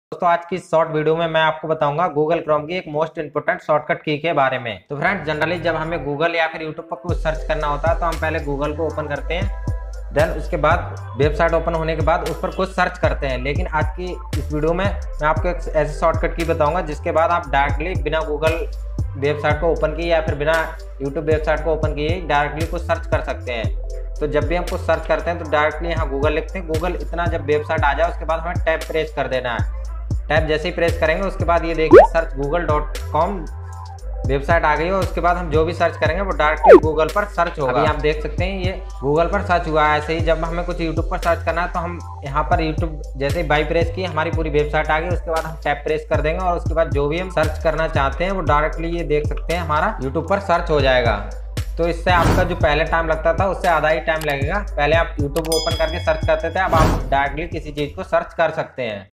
Google chrome most important shortcut key|| Google chrome most impotant shortcut key
Hello friends,
                 Is video me mene google chrome ki 1 most important shortcut key ke baare me bataya hai jo har chrome user ko pata hona chahiye.
agar aapko video pasand aaye to video ko like 👍 karna n bhule.

My Gears:
Mic cum earphone